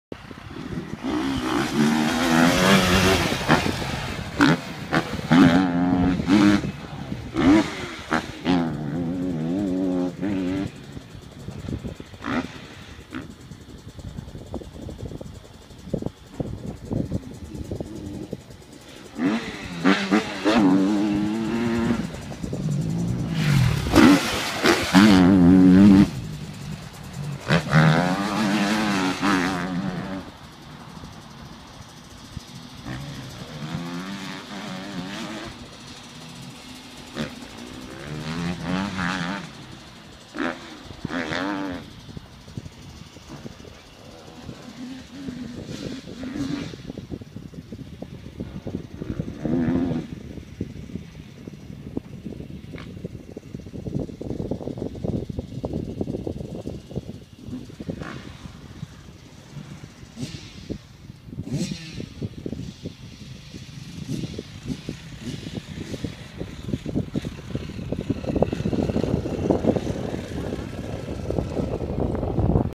MXGP 2014 Jose Butron 17# KTM 350cc (Sanlucar "el barrero") HD 1
El piloto del mundial Jose butron entrenando en el circuto del Barrero con su KTM 350cc